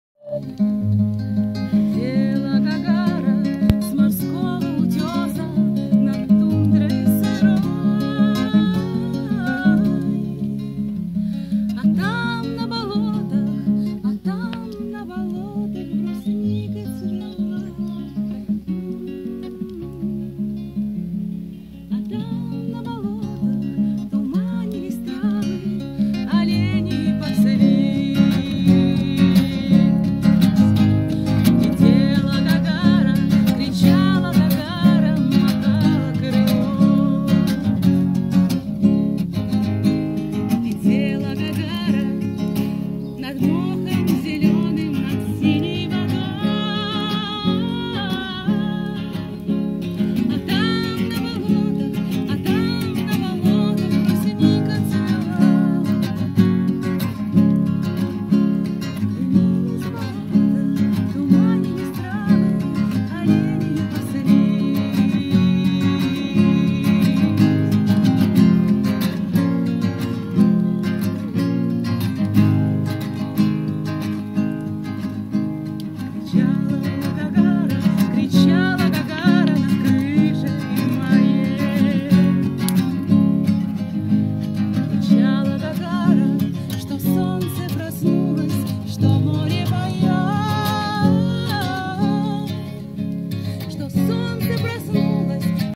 песни под гитару в веселой компании, Отдых в Подмосковье, PineRiver, Коттеджи
майские праздники, песни под гитару,